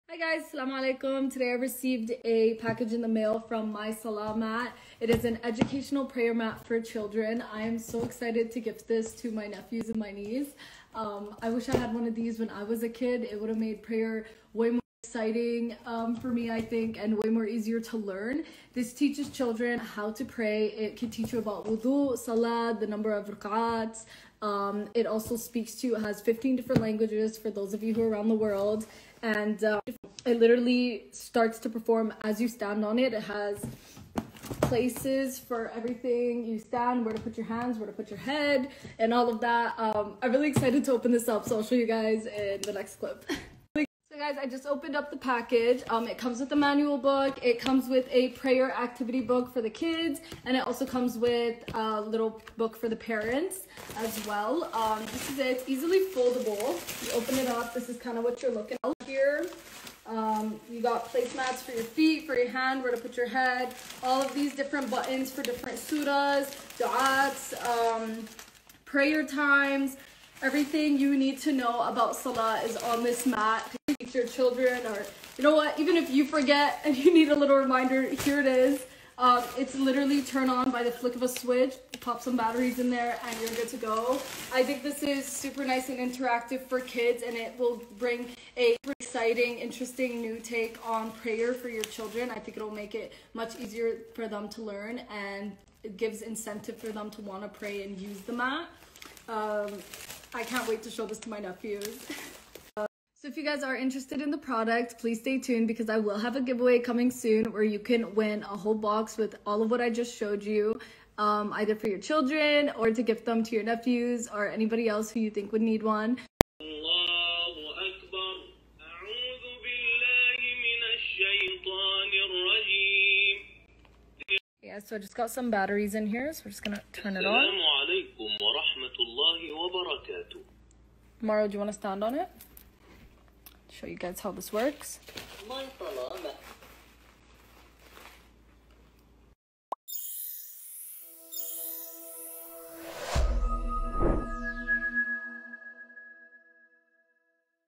My son can now pray because of this prayer rug 🥹
My Salah Mat is a Prayer Mat designed to educate children on how to perform the Muslim prayer in a fun and enjoyable way. The interactive prayer mat is touch sensitive and has pre-recorded keys that can be touched to reveal the sound, such as prayer times, how to do wudu, recite surahs, say duas, and much more. It can teach about the different times of salah, and the various bodily movements and positions, and what to say during them. In order to make the learning process easy, My Salah Mat will guide the user through two rakat of salah using a simple to understand format.